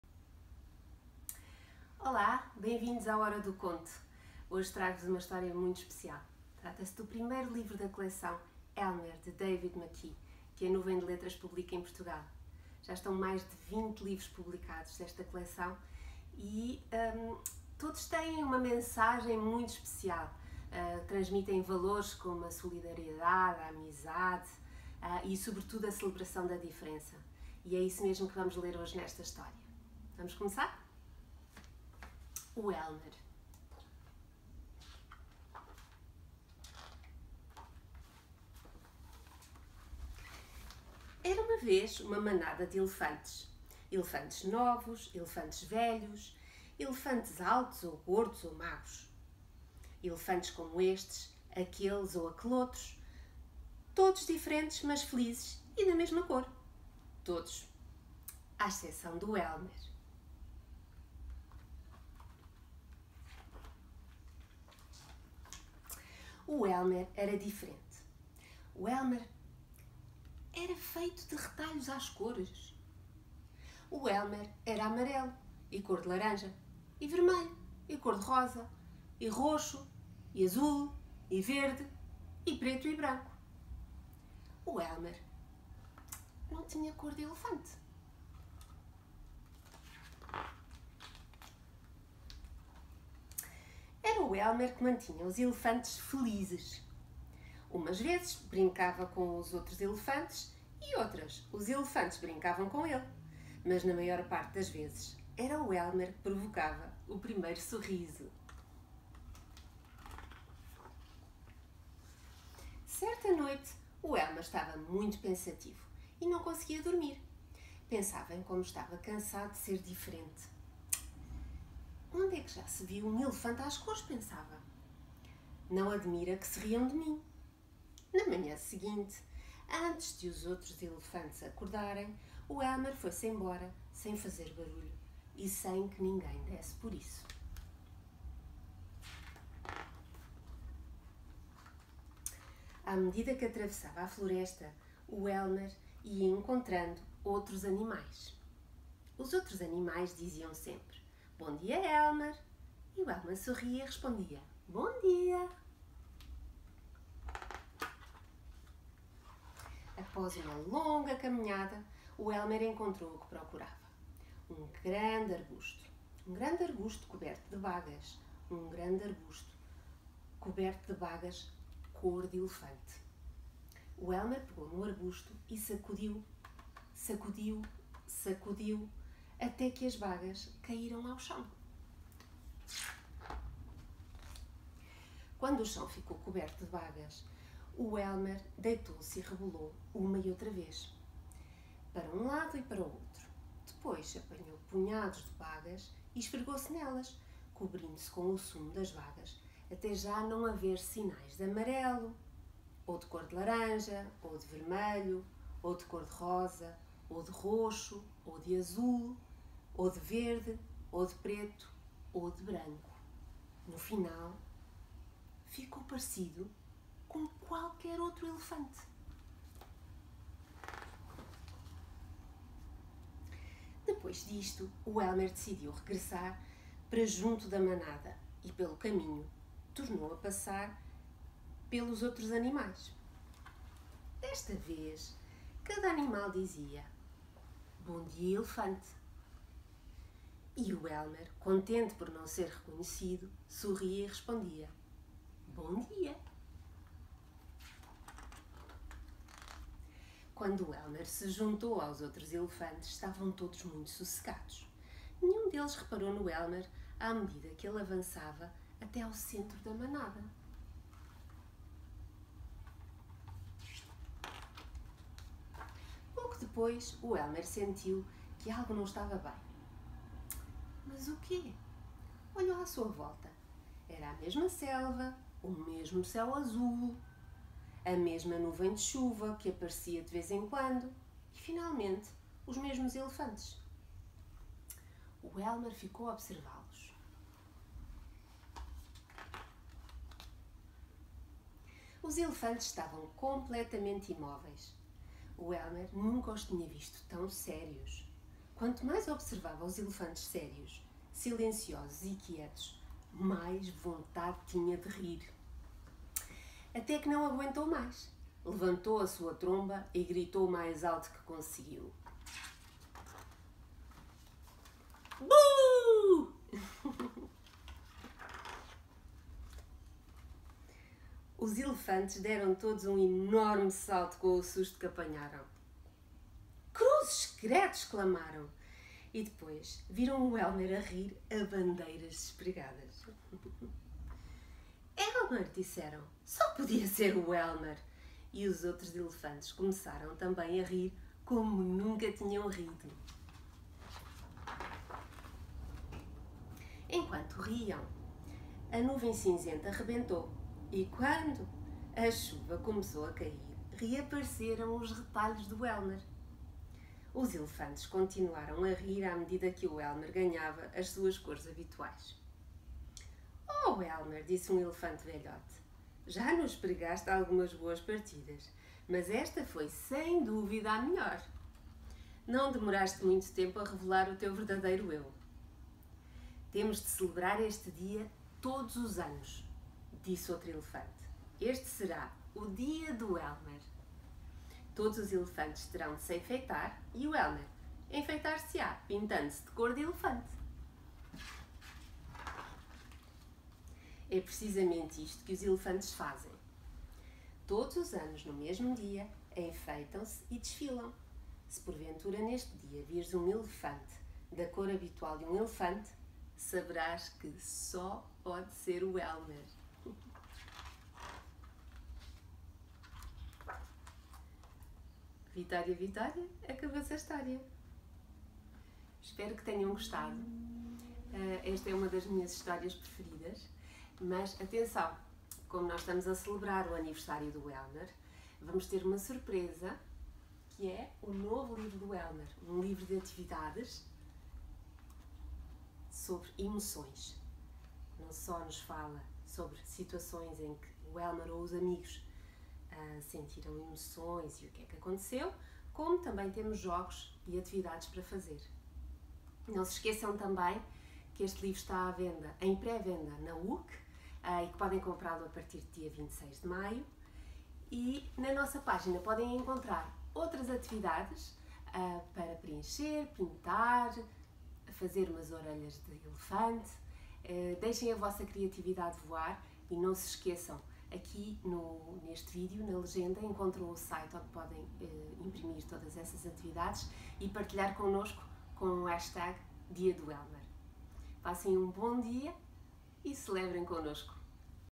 Hora do Conto: O Elmer, de David McKee
HORA DO CONTO: Hoje vamos descobrir a história do Elmer e porque é que a 23 de março o celebramos o DiadoElmer! Oiçam a história que a Joana Gonçalves tem para vos contar e visitem o para descarregarem as atividades que vos propomos esta semana!

Partilhem depois connosco com o
Edição Nuvem de Letras